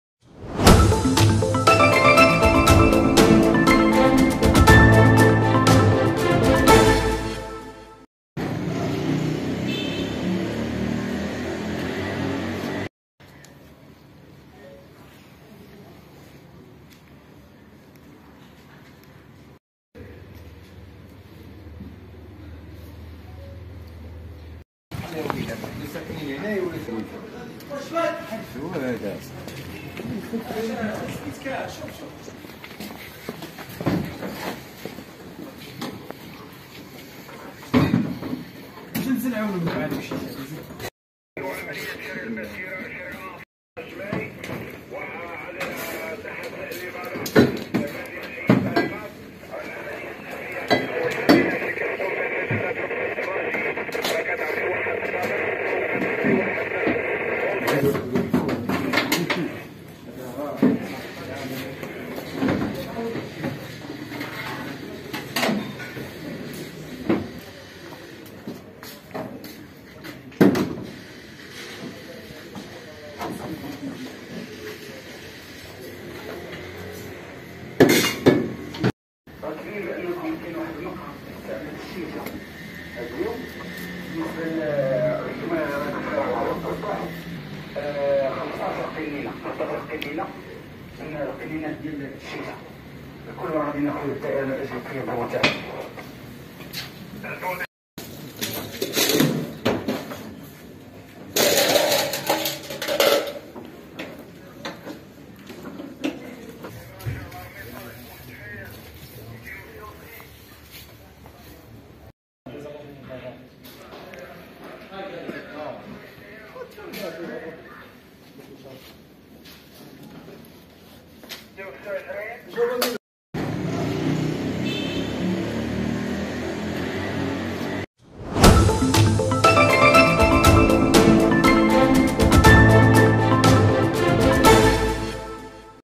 العثور على عبوات شيشة سرية في أول أيام رفع الحجر بالمدينة القديمة بمحل لبيع المواد الغذائية
العثور على زجاجات شيشة سرية في اول ايام رفع الحجر بالمدينة القديمة بمحل لبيع المواد الغذائية

# أخبار_عاجلة